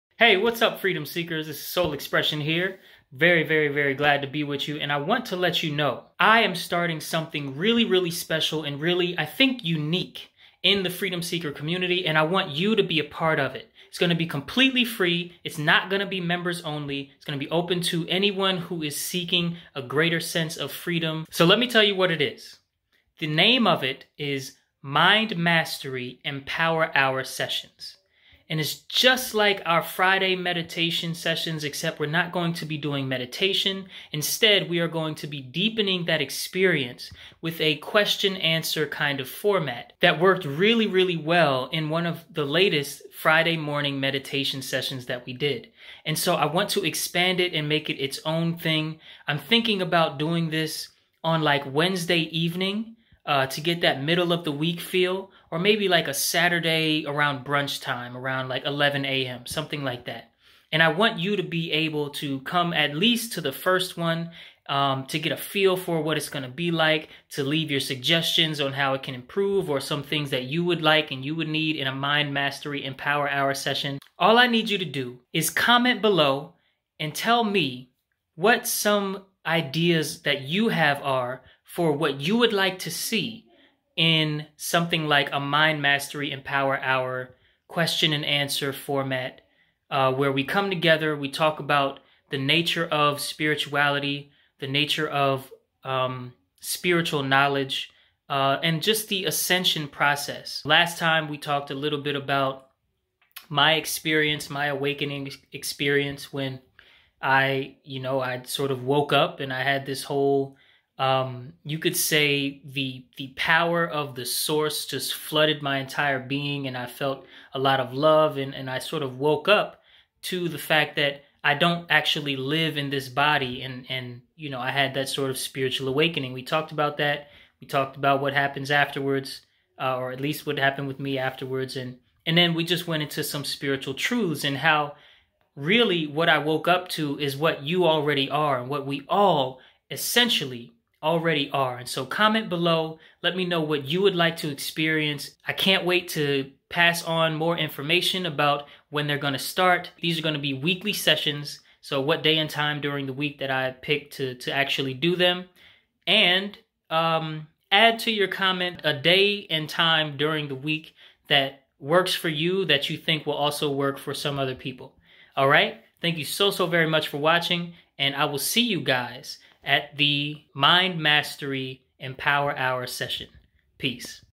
EmPower Hour Mind-Mastery Sessions starting next week
Welcome to a new era in your Freedom Seeker Journey!

The time has come for us to come together and put our fears and confusions to rest, for good.

Starting next week, we will be opening the door to all Freedom Seekers around the world to a unique opportunity for self-realization.

I am so glad to announce that I’m starting something new that has the potential to completely change your life.

Announcing: Mind Mastery “EmPower Hour” Sessions with Sol Xprsn

If you have ever had questions about how to assimilate spirituality into your daily life consistently...

How to keep your Truth when times get tough or people get difficult...

How to remember that things will be okay when they seem to be falling apart and you’ve run out of moves…